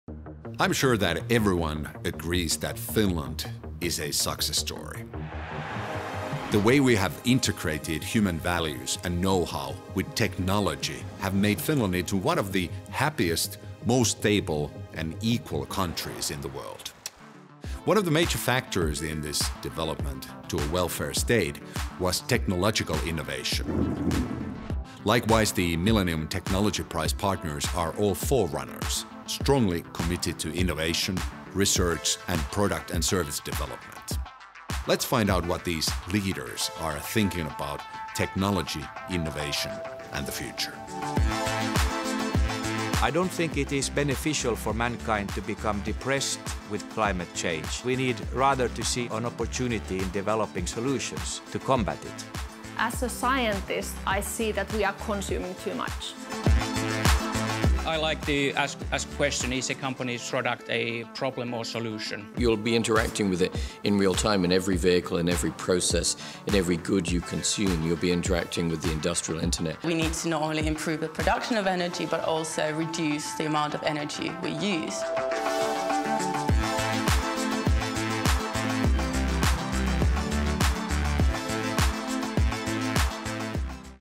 Millennium Talks, with innovation leaders teaser video
Together with our Partners we launch Millennium Talks with innovation leaders video series concentrating on the extensive impact of science-based technological innovations on the well-being of the society and its citizens. The host Henkka Hyppönen interviews the Millennium Technology Prize partners about their solutions for the greatest challenges of our time. Watch the teaser!
There will be a new episode published every week until the beginning of April! The first episode will be published during the week 8.

Archive material by Yle (Fennada-Filmi, Filmiseppo and Veikko Itkonen) published under CC-BY 4.0 license.